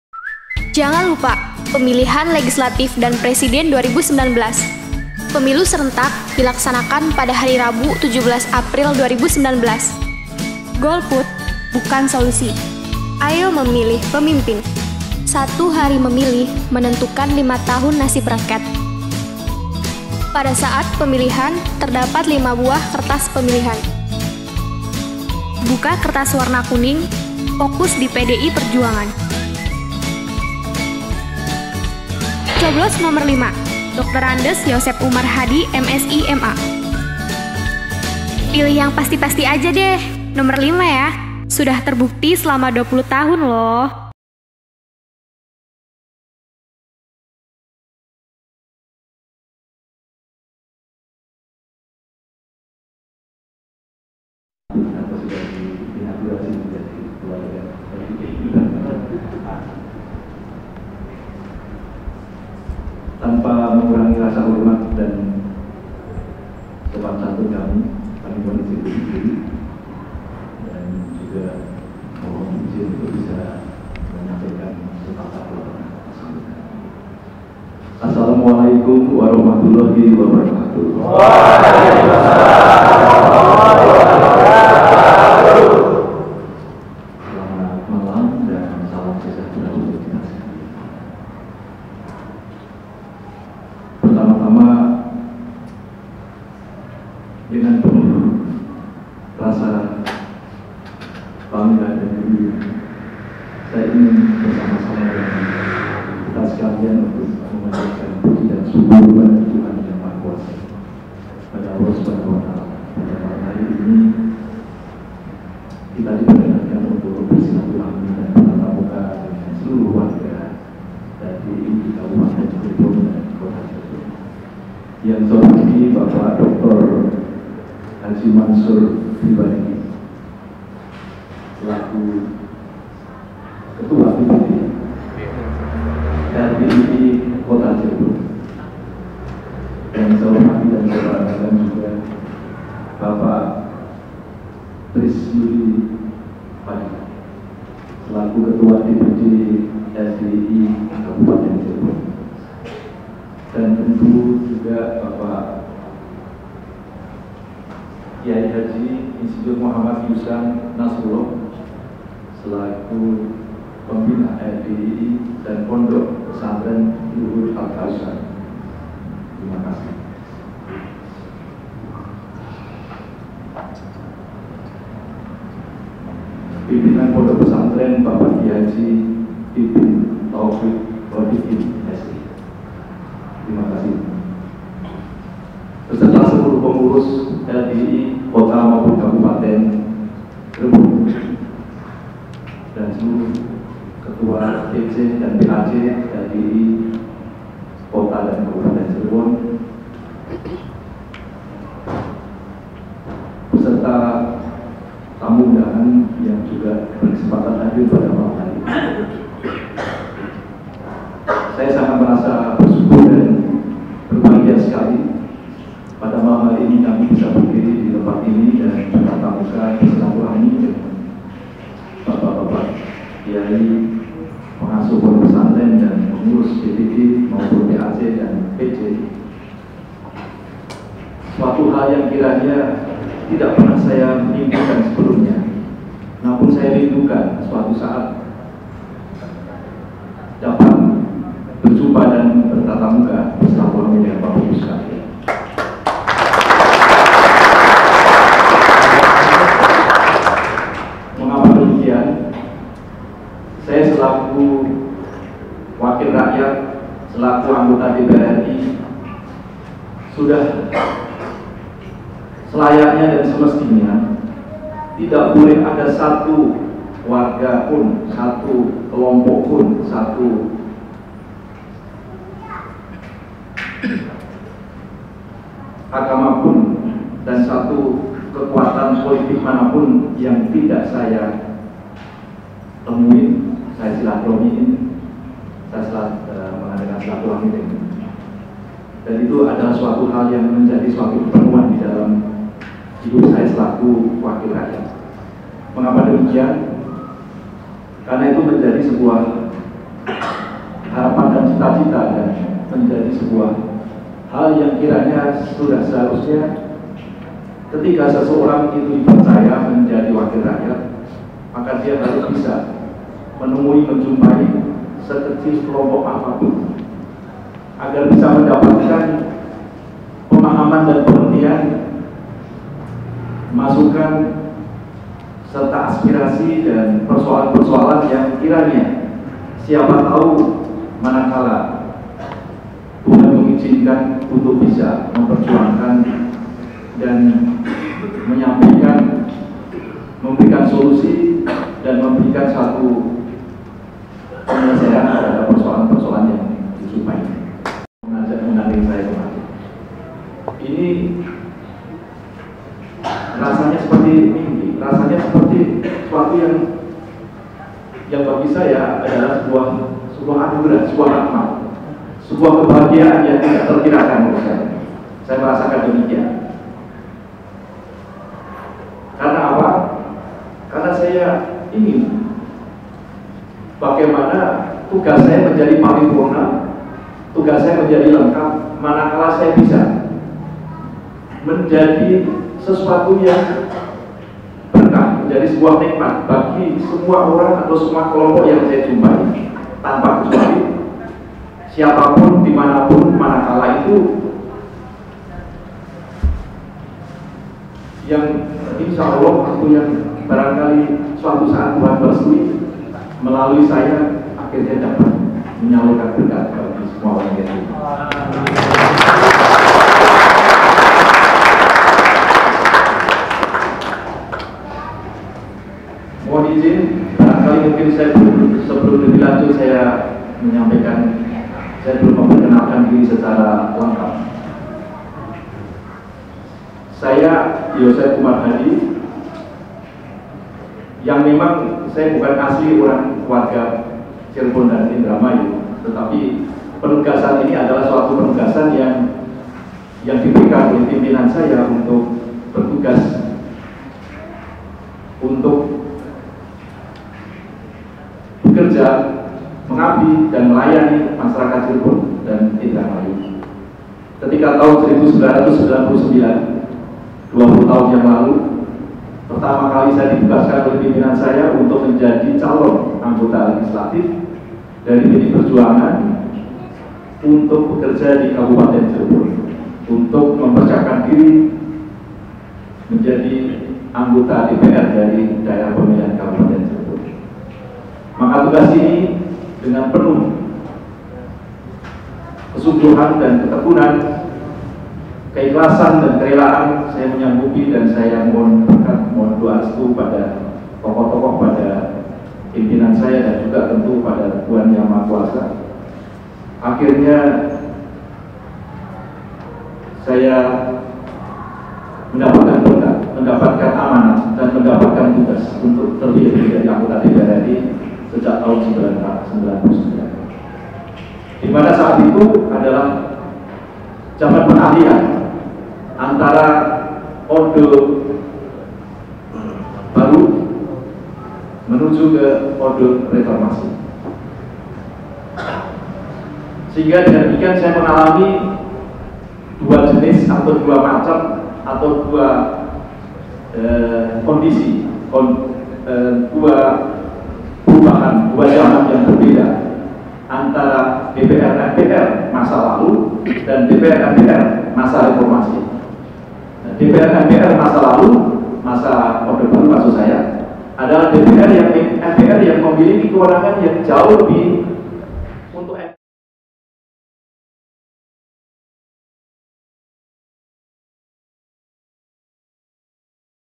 Bersilaturahmi dengan keluarga besar LDII Cirebon
Bersilaturahmi dengan keluarga besar LDII (lembaga Dakwah Islam Indonesia) Cirebon yaitu di Pondok pesantren Luhur Al-Kautsar.